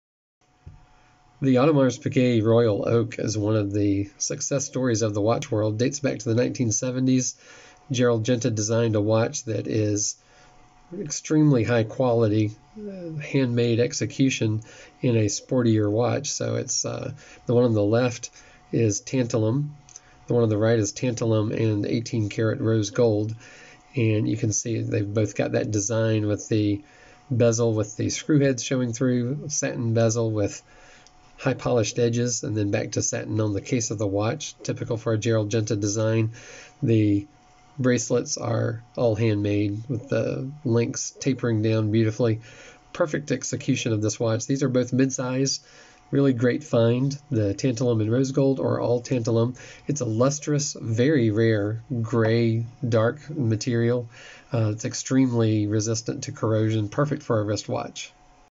Audemars Piguet Royal Oak Midsize 33mm Tantalum Mens Watch | SwissWatchExpo
The Audemars Piguet Royal Oak is famous for being the world's first luxury sports watch made in steel. These two models are made of a metal called tantalum - a rare, hard and  lustrous metal that is highly corrosion-resistant. Choose between a monochromatic tantalum model with a grey Tapisserie dial, or add a touch of shine with 18k rose gold.

Check out our collection of Audemars Piguet watches for men and women at SwissWatchExpo.

Featured watch (prices may change):
Audemars Piguet Royal Oak Midsize 33mm Tantalum Mens Watch

Audemars Piguet Royal Oak Tantalum Rose Gold Midsize Mens Watch 56175

Transcript:

The Audemars Piguet Royal Oak is one of the success stories of the watch world. Dates back to the 1970s, Gerald Genta designed a watch that is extremely high quality, handmade execution in a sportier watch. So it's the one on the left is tantalum ; the one of the right as tantalum and 18 karat Rose gold. And you can see they've both got that design with the bezel, with the screw heads showing through the satin bezel with high-polished edges and then back to set. And on the case of the watch, typical for a Gerald Genta design, the bracelets are all handmade with the links tapering down. Beautifully perfect execution of this watch. These are both mid-sized really great. Find the tantalum and Rose gold or all tantalum. It's a lustrous, very rare gray dark material. It's extremely resistant to corrosion. Perfect for a wristwatch.